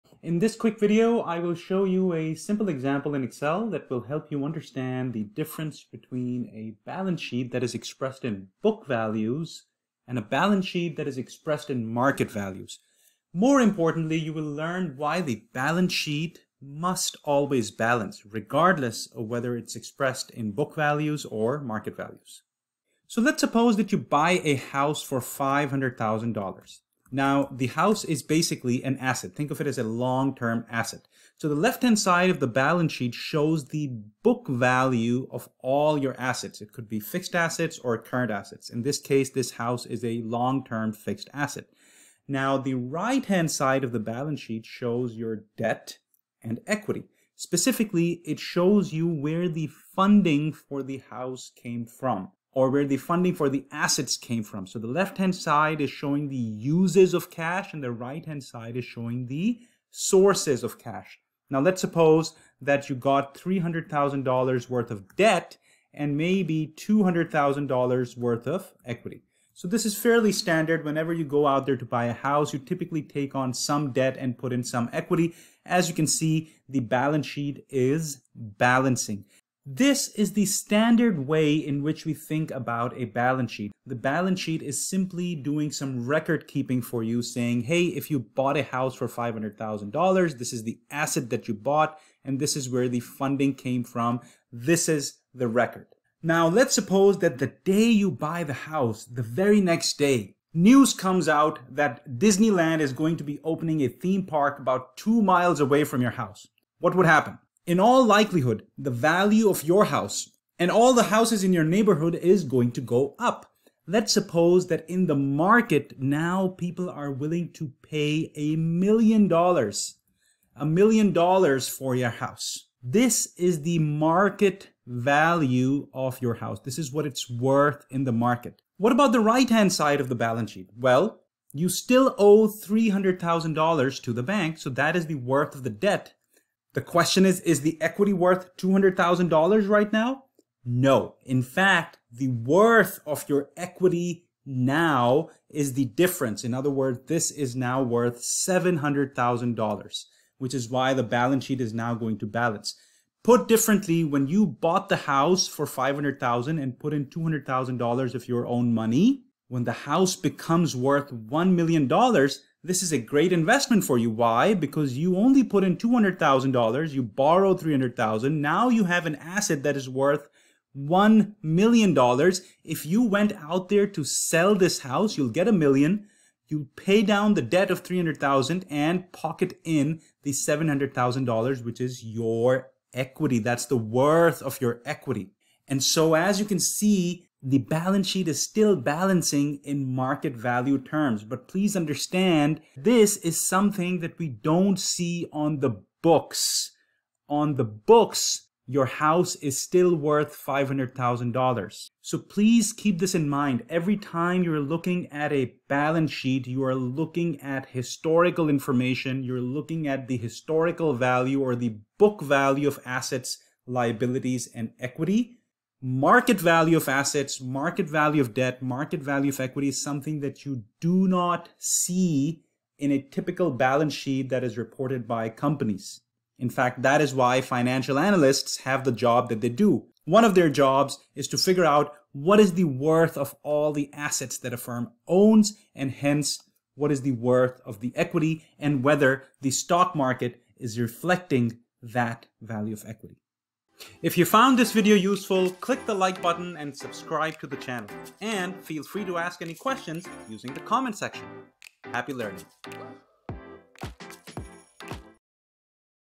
Book Value vs Market Value Balance Sheet: Simple Example Using Excel
In this video, I show a simple example in Excel to illustrate the difference between a balance sheet expressed in book values and a balance sheet expressed in market values. More importantly, I show that the balance shet identity holds in both book value and market value terms.